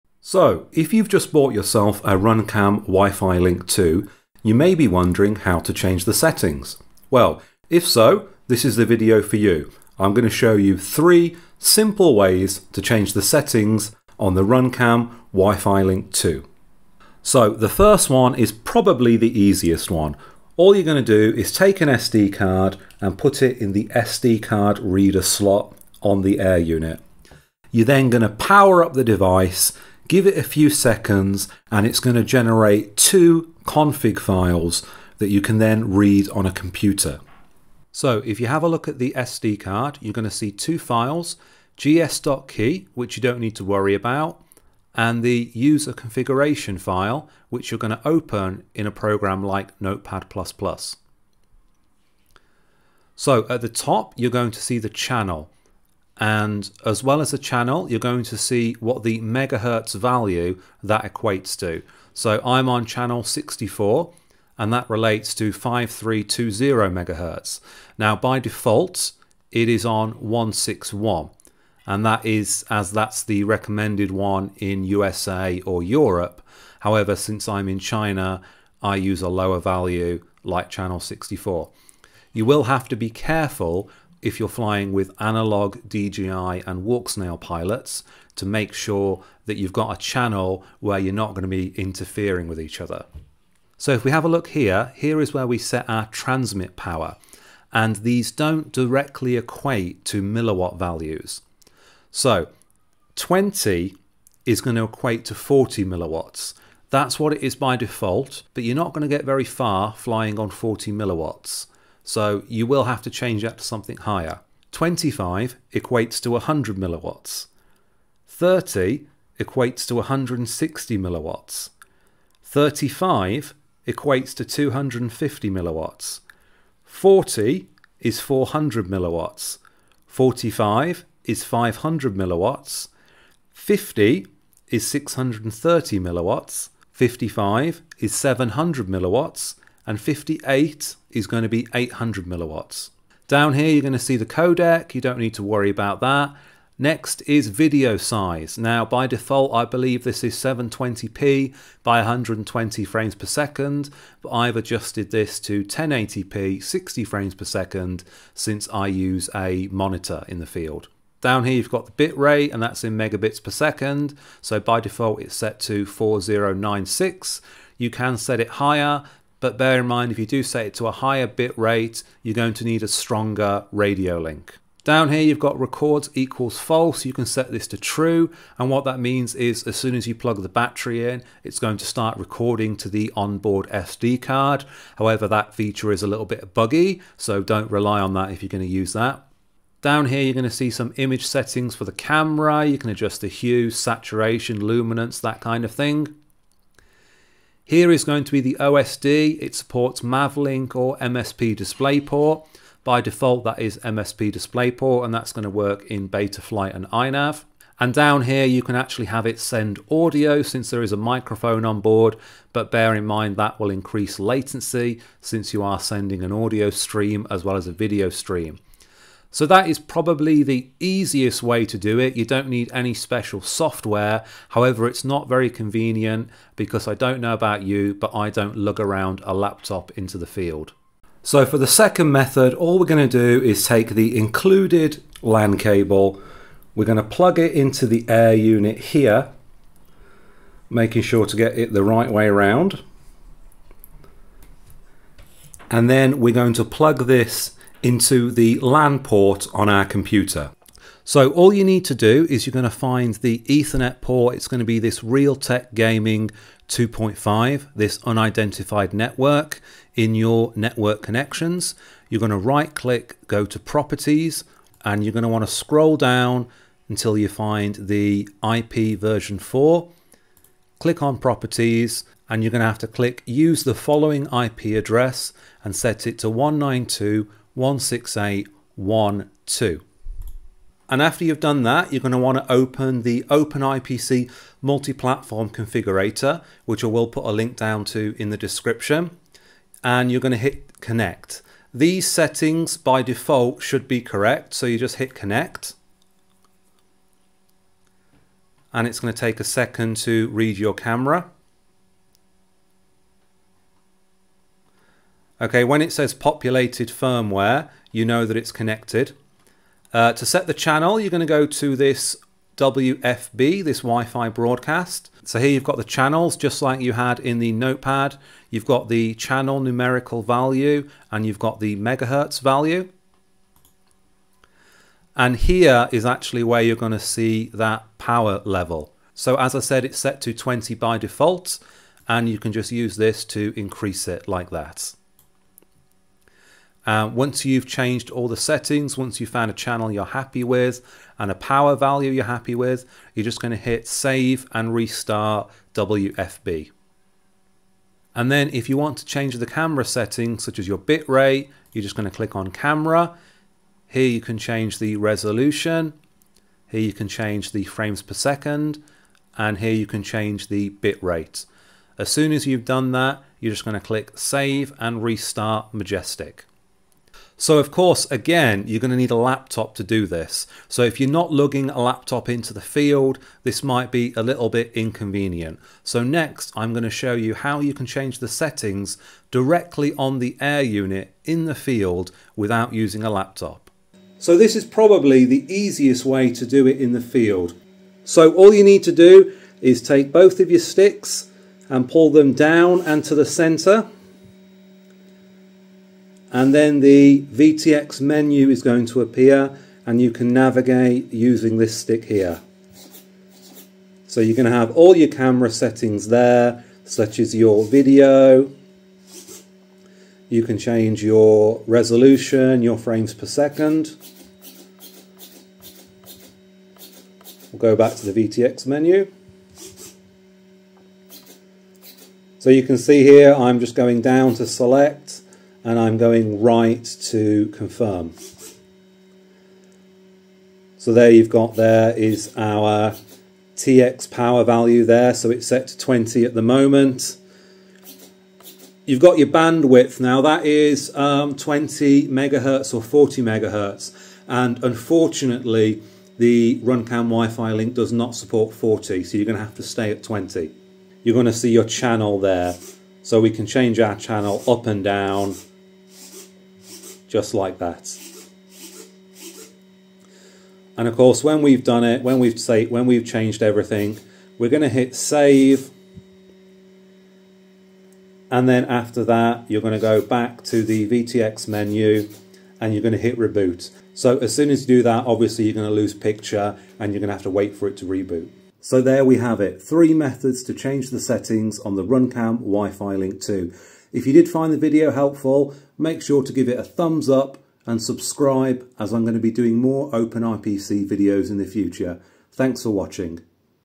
How to change settings on the Runcam WiFiLink 2 - OpenIPC Tutorial
In this OpenIPC tutorial video I take a look at how to change the settings on the Runcam WiFiLink 2 with 3 easy methods.